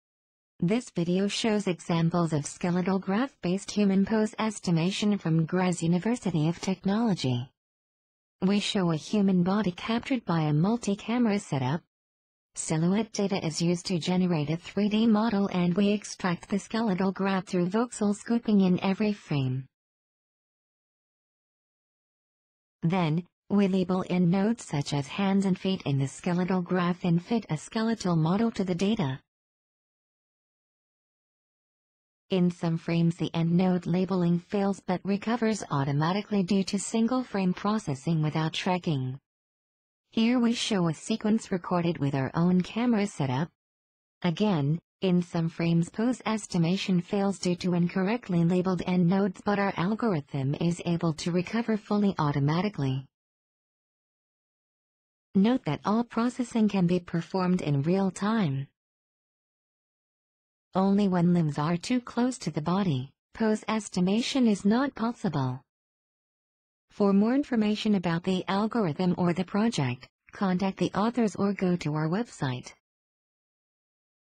Skeletal Graph Based Human Pose Estimation in Real-Time
This is the supplemental video (won the Best Supplemental Material Award) of the publication:

Matthias Straka, Stefan Hauswiesner, Matthias Rüther and Horst Bischof: Skeletal Graph Based Human Pose Estimation in Real-Time. Proceedings of the British Machine Vision Conference (BMVC), 2011

Institute for Computer Graphics and Vision (ICG)
Graz University of Technology

We propose a new method to quickly and robustly estimate the 3D pose of the human skeleton from volumetric body scans without the need for visual markers. The core principle of our algorithm is to apply a fast center-line extraction to 3D voxel data and robustly fit a skeleton model to the resulting graph. Our algorithm allows for automatic, single-frame initialization and tracking of the human pose while being fast enough for real-time applications at up to 30 frames per second. We provide an extensive qualitative and quantitative evaluation of our method on real and synthetic datasets which demonstrates the stability of our algorithm even when applied to long motion sequences.